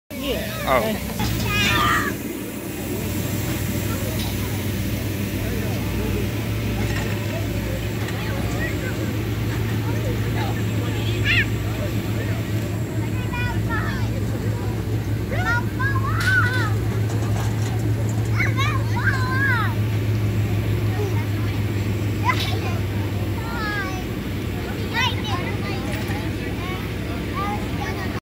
Hang On!!! Mechanical Bull Riding at the 2018 Bland County Fair
Bland County Fairgrounds   Bland, VA